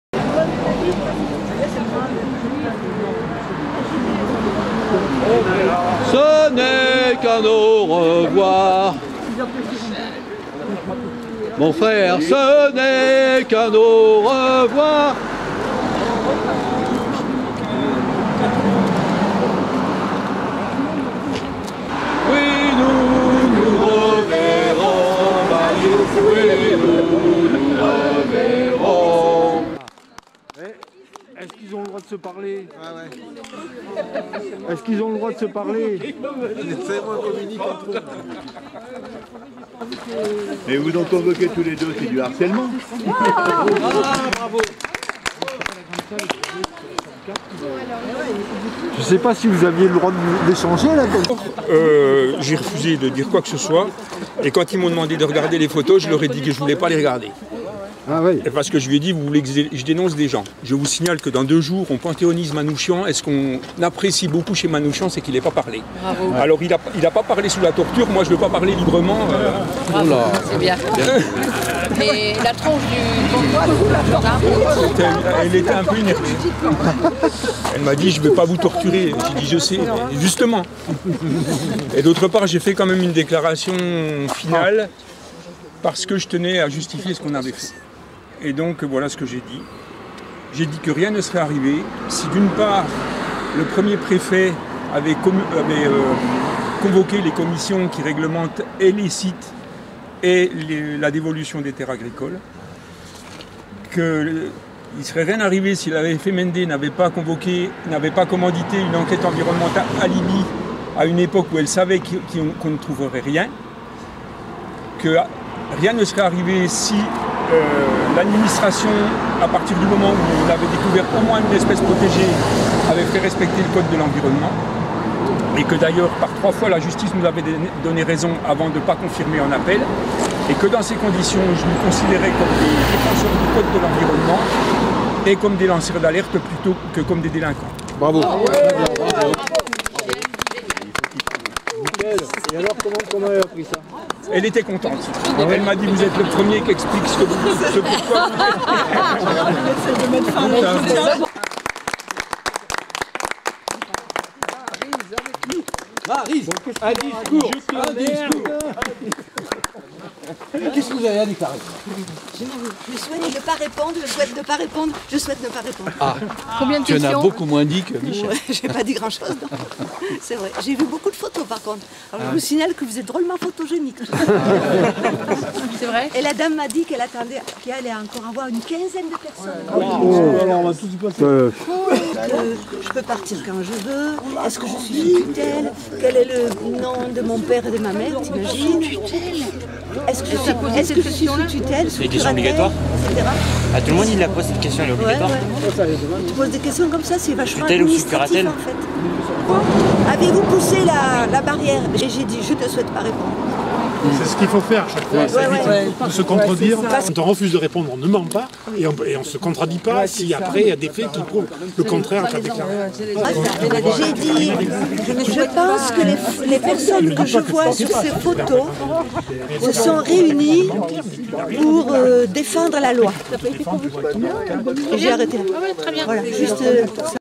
convoqués Maryse et Michel
Gendarmerie de Thueyts suite aux manifs contre la "basilique" de St Pierre les convocations vont bon train...
Beaucoup sont solidaires et présent quand ils peuvent...
Pour la plupart ce sera peut-être une amende.
(ce n'était pas une manif d'agriculteurs! dommage! )